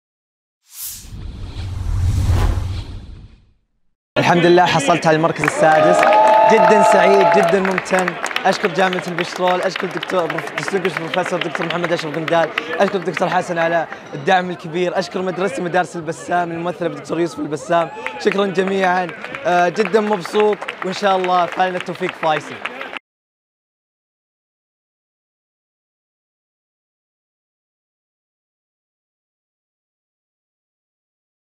الطالب: عبدالله الحمدات الفائز بالمركز السادس بالجوائز الكبرى في إبداع 2022
مشاعر السعادة ينقلها لنا ولجامعة الملك فهد للبترول والمعادن بفوزه بالمركز السادس بالجوائز الكبرى في إبداع_2022..

الطالب: عبدالله الحمدات